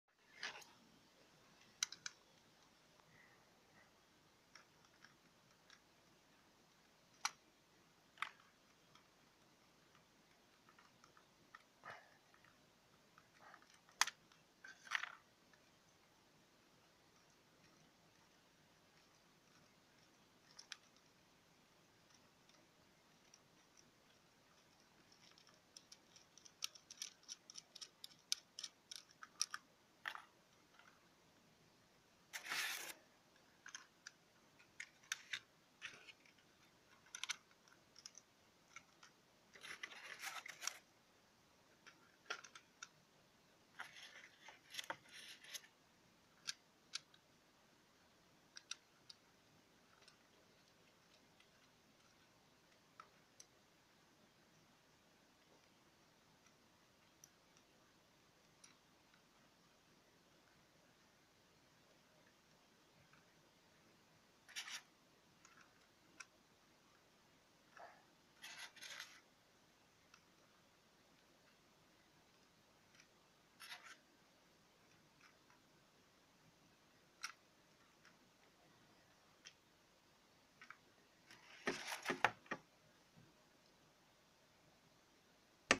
Changing the mechanism on vinyl clock
It is a video-lesson about broken clock. In this video you can see how to change mechanism if clock's hands were broken.